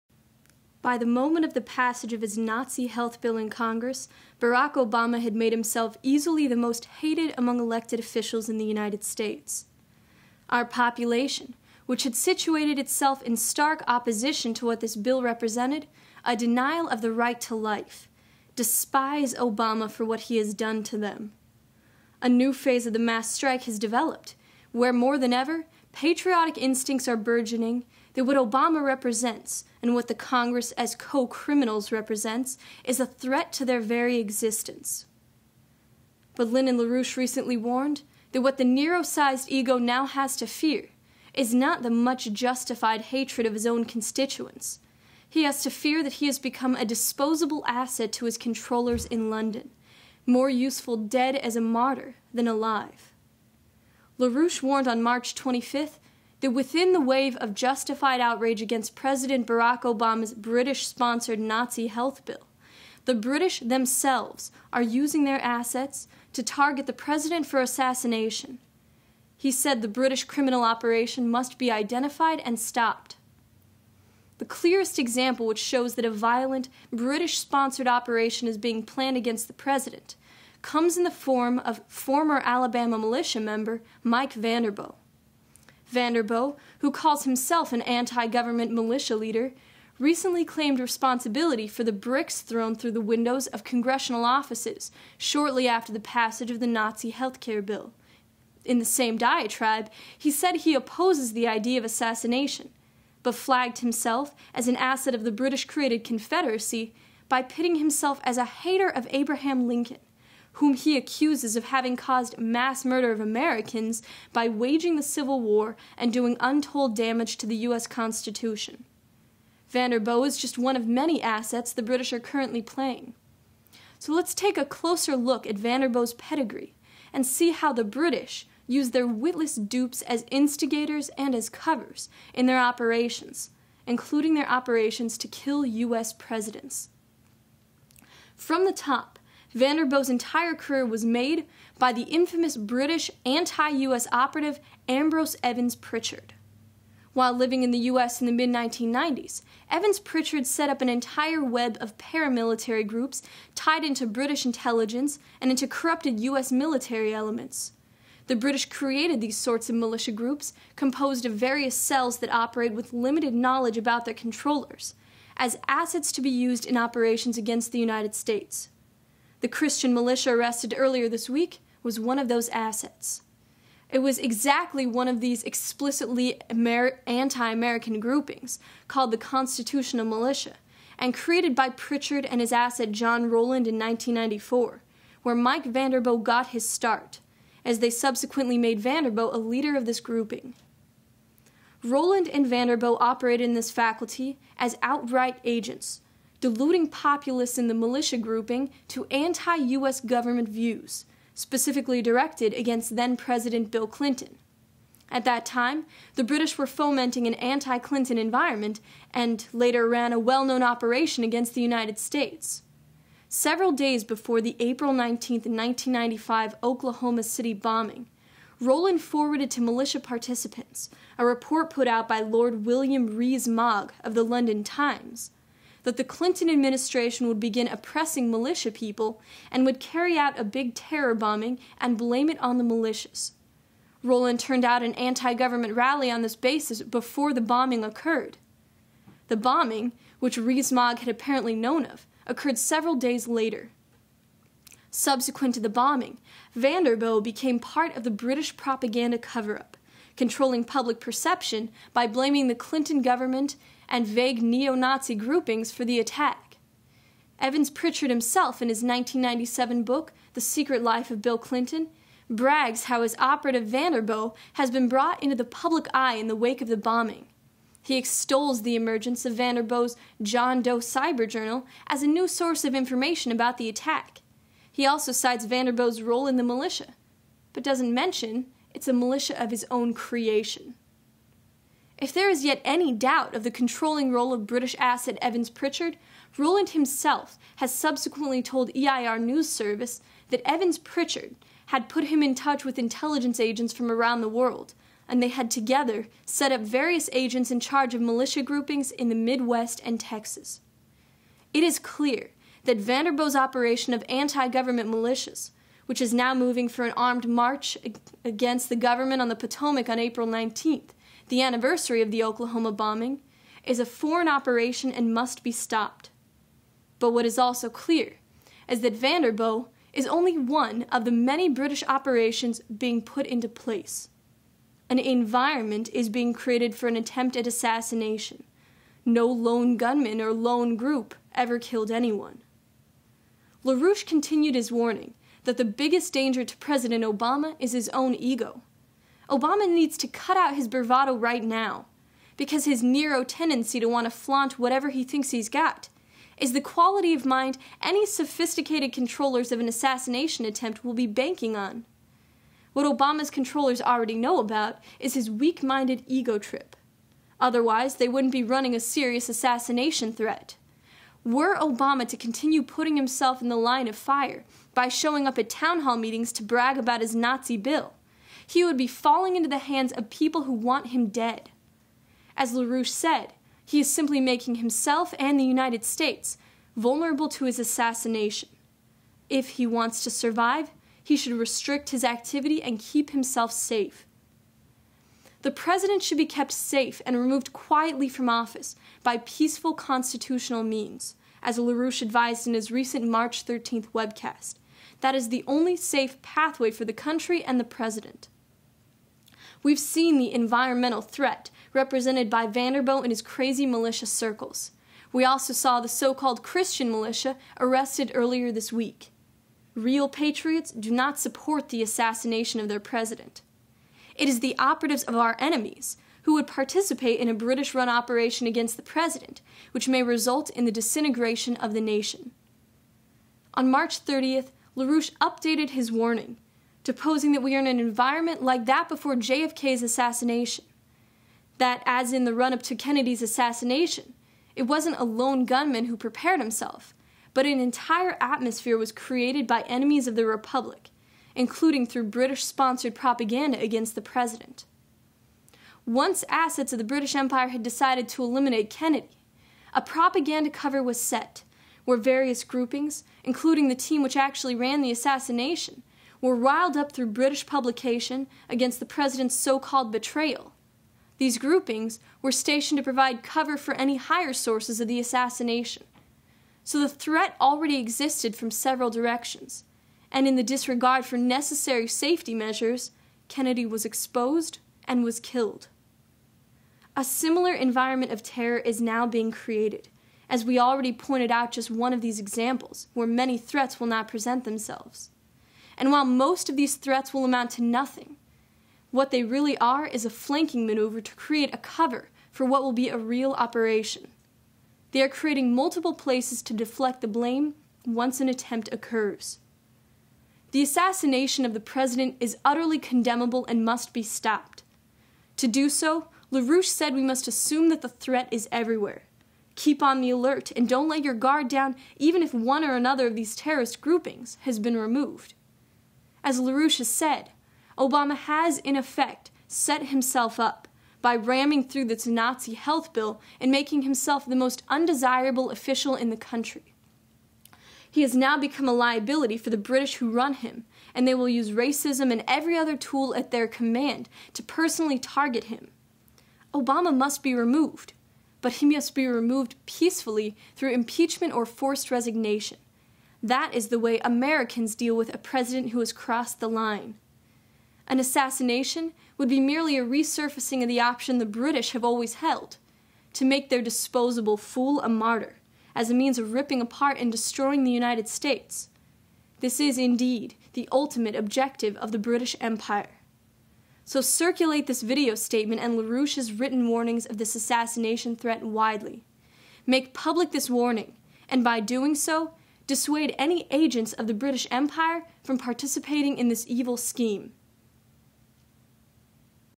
Stop the British Assassination Plot
-- We need Obama impeached, NOT assassinated.  The present situation shows signs of the preparation for a British directed assassination of the president, in order to throw the United States into chaos and ungovernability.  This must be exposed and stopped.